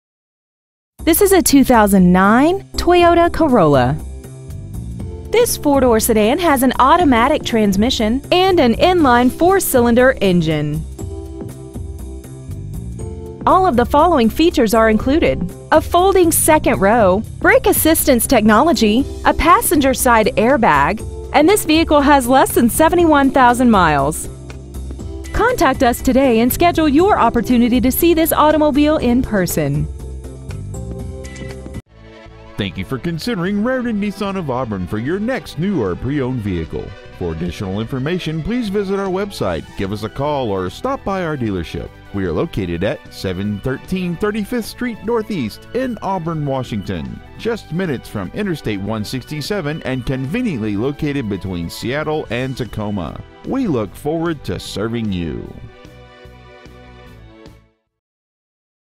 Preowned 2009 Toyota Corolla Auburn WA 98002
Year : 2009
 Make : Toyota
 Model : Corolla
 Engine : 1.8L I4 DOHC 16V
 Trans . : Automatic
 Exterior : Gray
 Miles : 69,266
 Interior : Gray
 Stock : 92052707